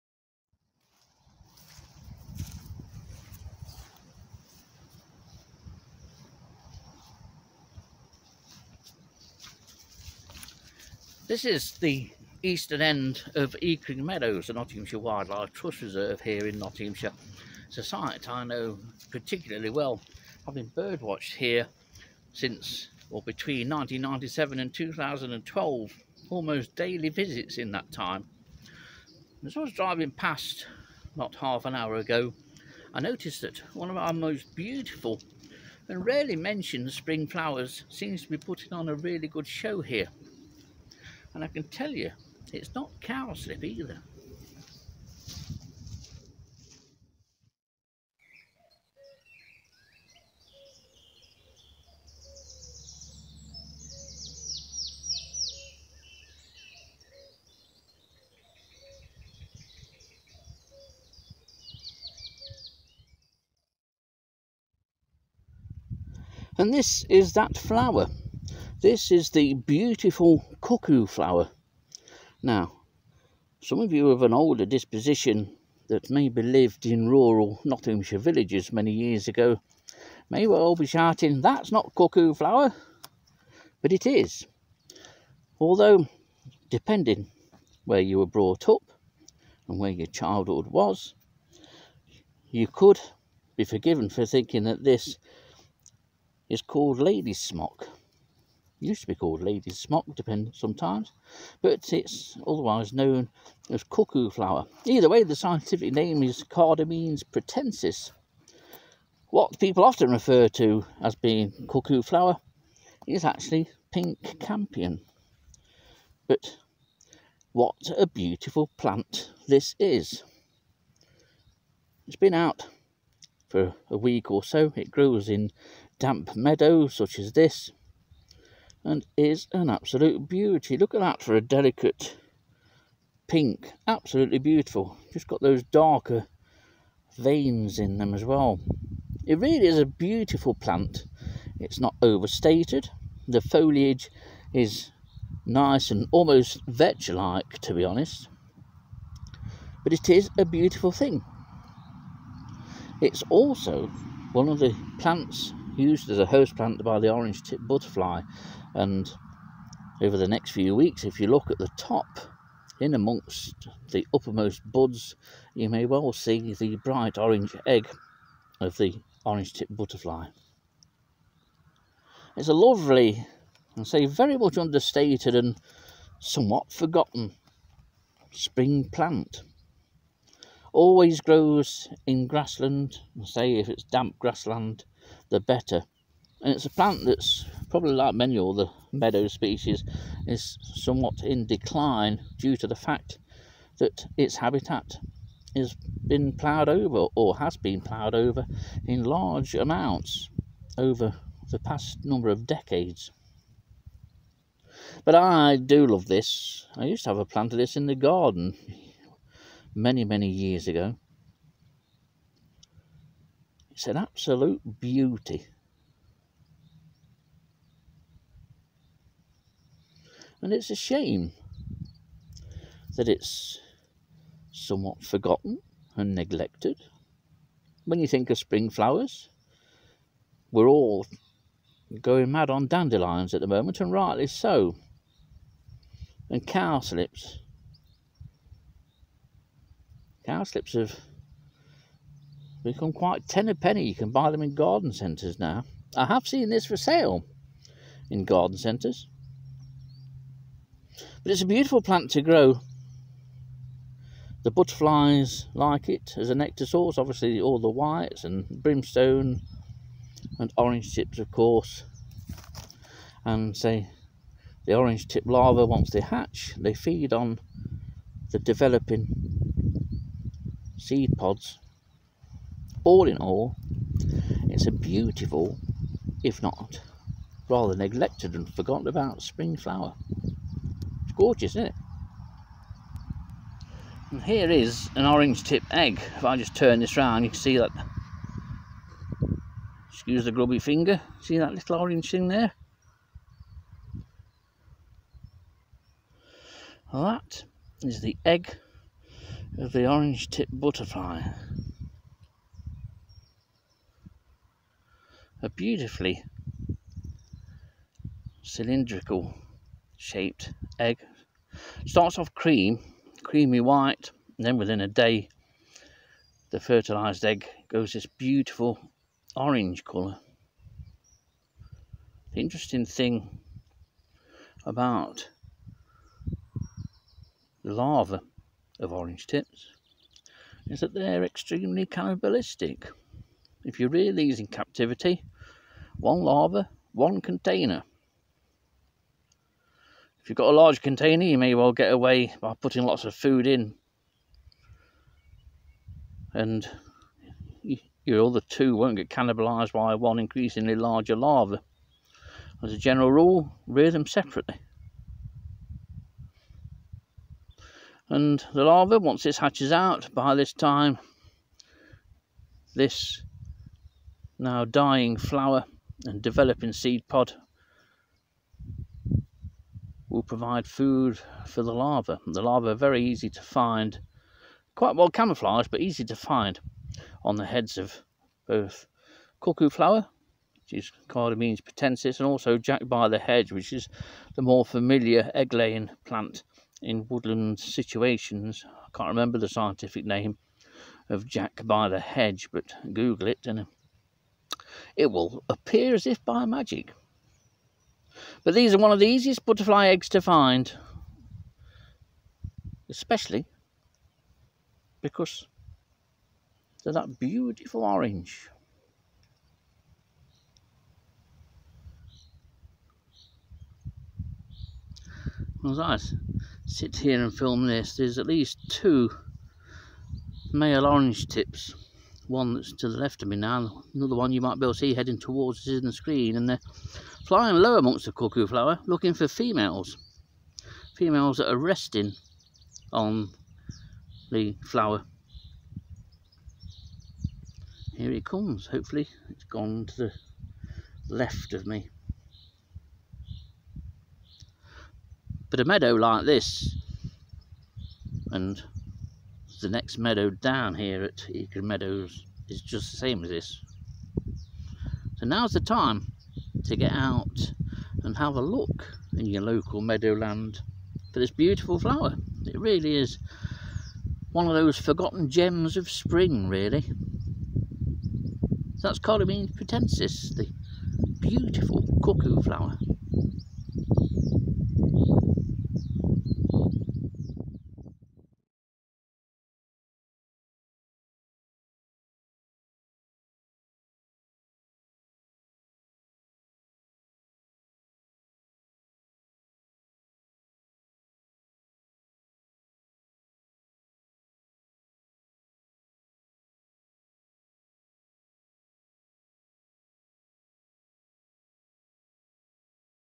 A neglected Spring flower and the Orange Tip egg
One of our most delightful Spring flowers, which has declined over the years through the draining and ploughing over of most of our wet meadows. Once an easy flower to see, now it takes a bit more effort.